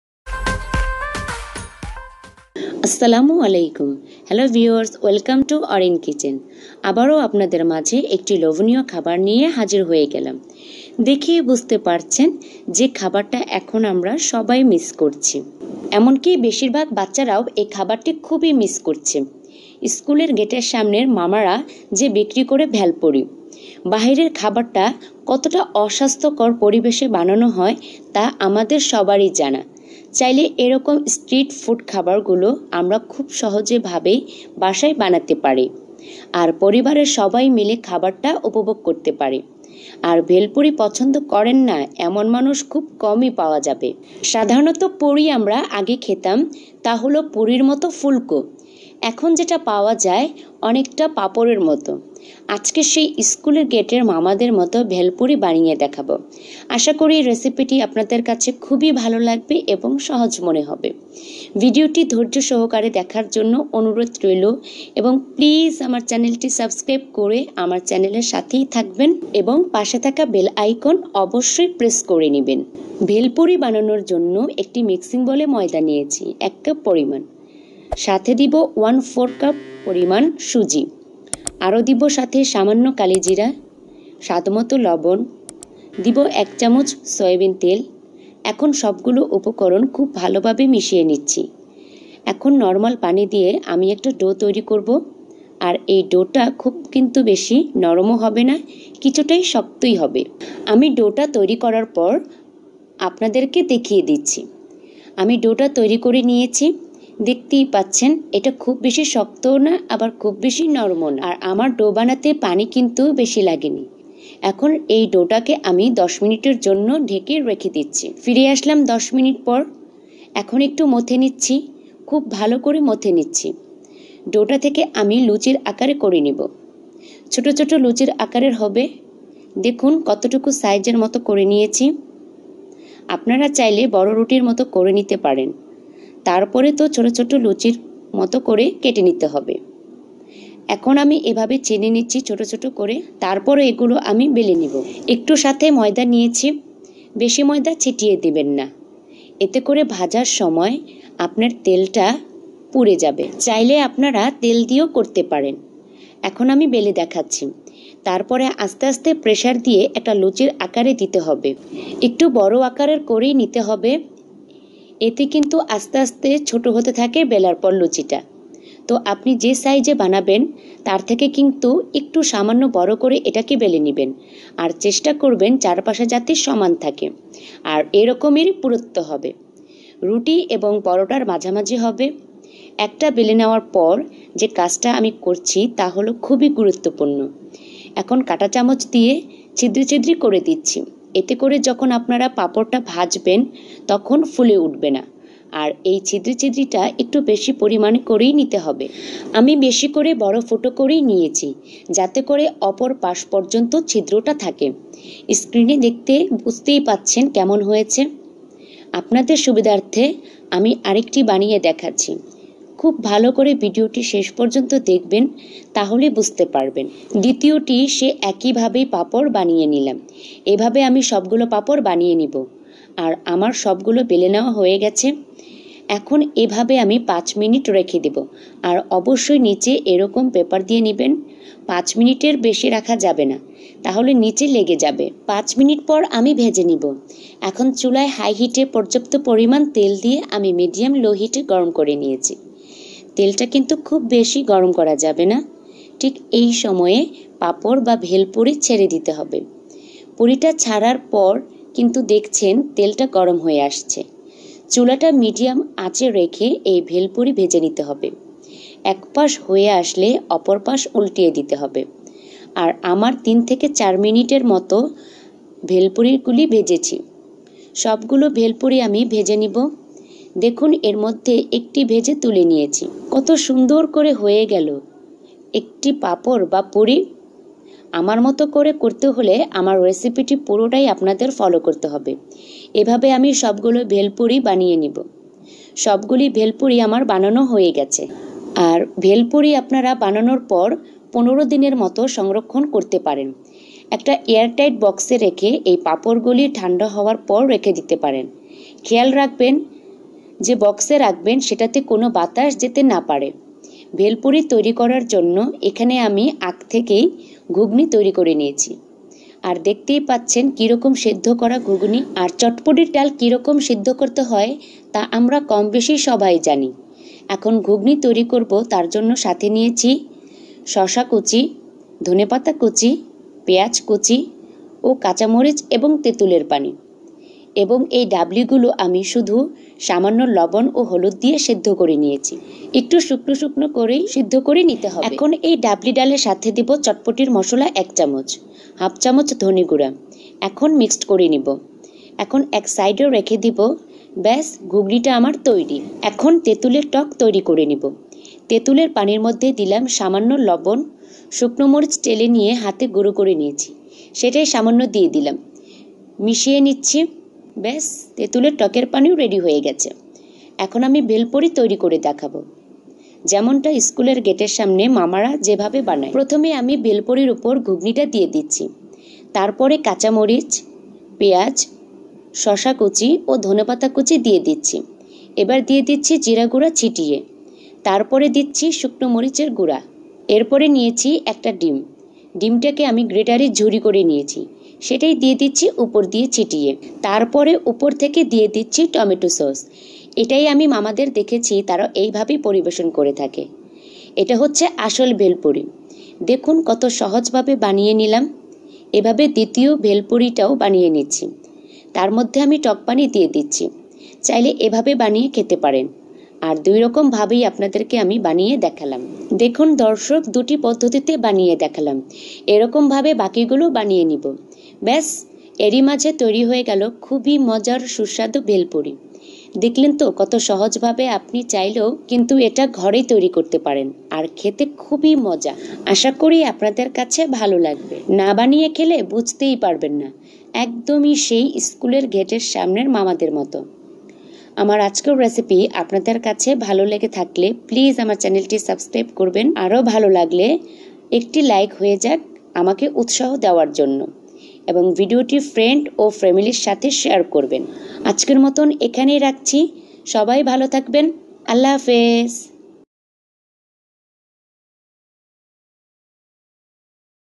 ইস্কুলের গেটের সামনের মামাদের বানানো সেই লোভনীয় মজার ভেলপুরি রেসিপি। সংরক্ষণ পদধতি সহ এখনি দেখে নিন
আসসালামু আলাইকুম💖কেমন আছেন সবাই💖এই ঈদে সবাই কে জানা  আন্ত্রতরিক  শুভেেচ্ছা।